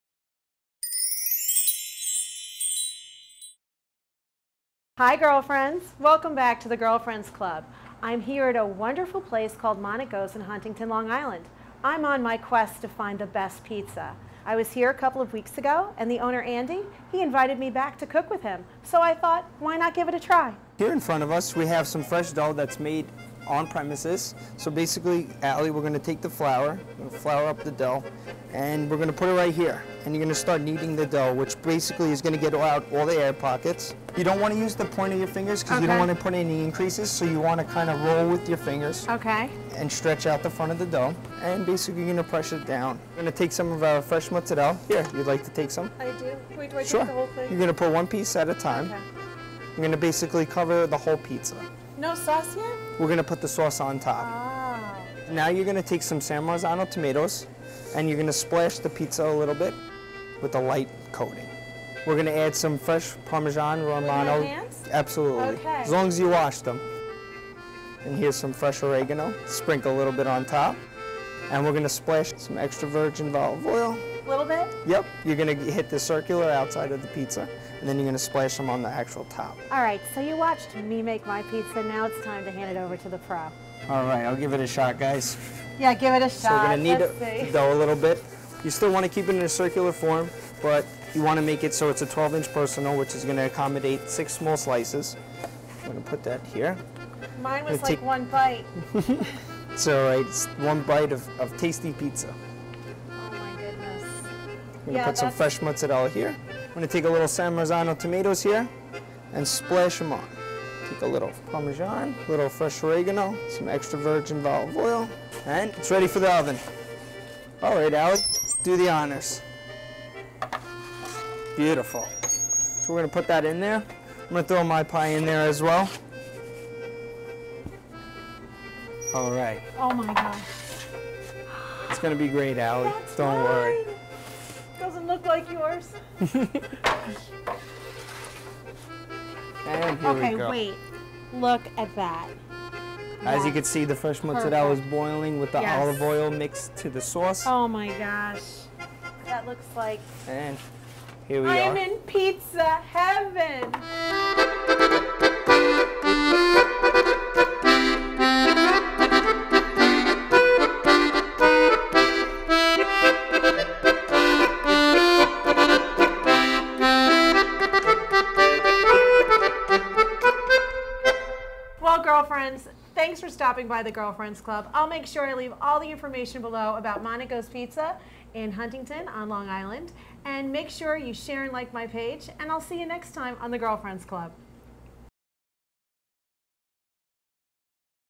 PIZZA! I am on the quest for the best pizza...
Pizza, its one of America's favorite foods! I am on the quest to find the best pizza. Living in New York gives me a great advantage...however, I have had amazing Pizza in Chicago, Miami and Los Angeles. If you have a favorite pizza place please leave the name and location below, maybe it will be my next stop!
Keep Sparkling Girlfriends!
Monaco's Pizzeria
Tell Andy I sent You!